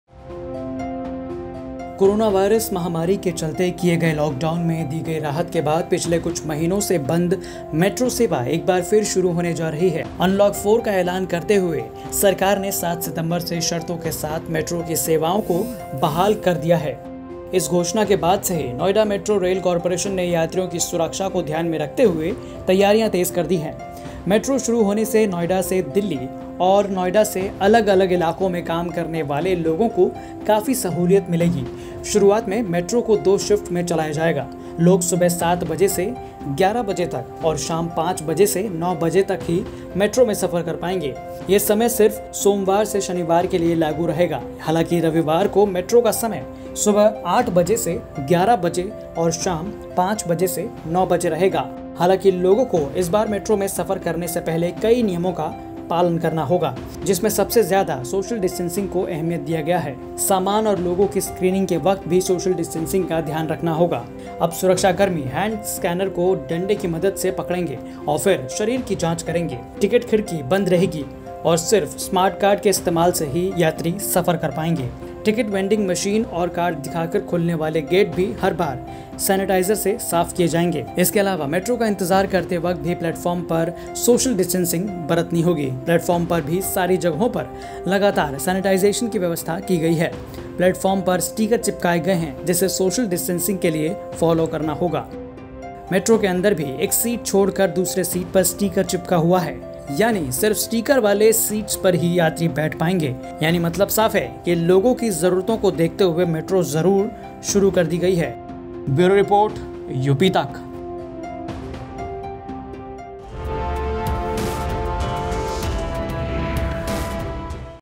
Unlock 4.0: 7 सितंबर से यूपी में मेट्रो शुरू, लेकिन सफर इतना आसान नहीं!
अनलॉक-4 का ऐलान करते हुए सरकार ने 7 सितंबर से शर्तों के साथ मेट्रो की सेवाओं को बहाल करने का ऐलान कर दिया है. इस घोषणा के बाद से ही नोएडा मेट्रो रेल कॉर्पोरेशन ने यात्रियों की सुरक्षा को ध्यान में रखते हुए तैयारियां तेज कर दी हैं.